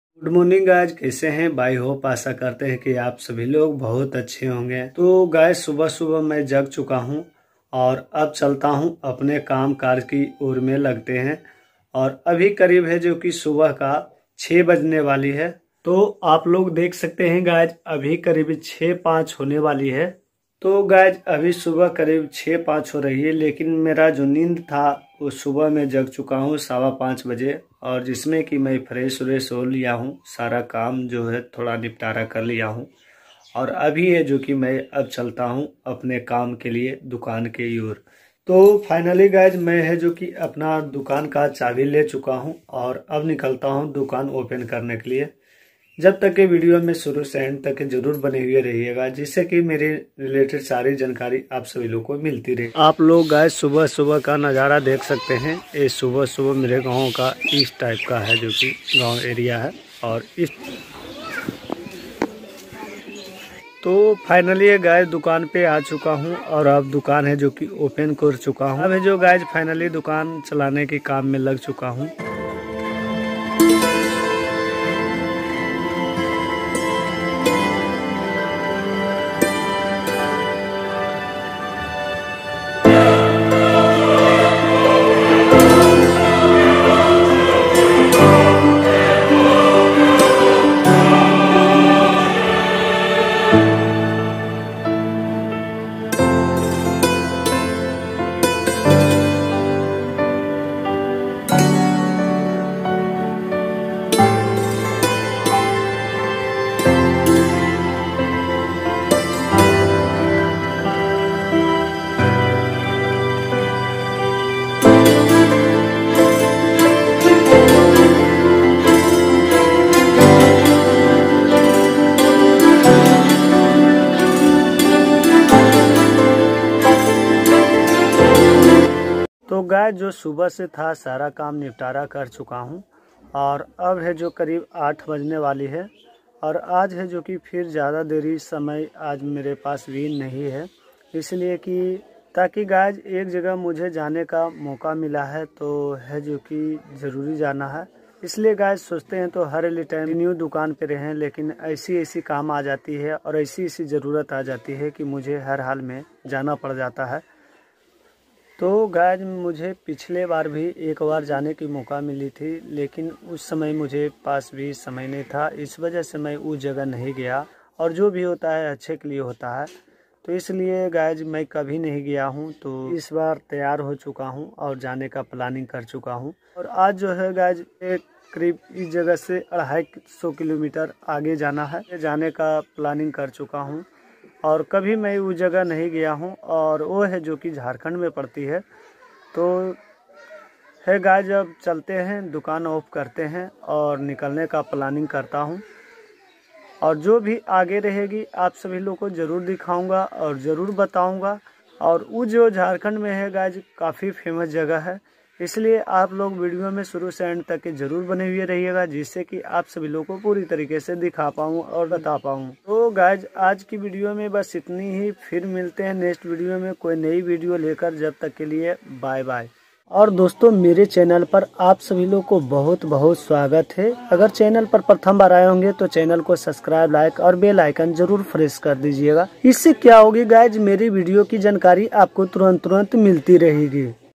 इस दूकानदार का असलियत जाने ?
doston is video mein aap Sabhi Logon Ko Gaon Se Lekar Bajar tak ki marketing ka video dikhaya hun

hope you will be good and happy.
|  उम्मीद है आप अच्छे और खुश होंगे।

Vijay Raj Vloge tour video

Vijay Raj Vloge video

Vijay Raj Vloge New video Bajar ka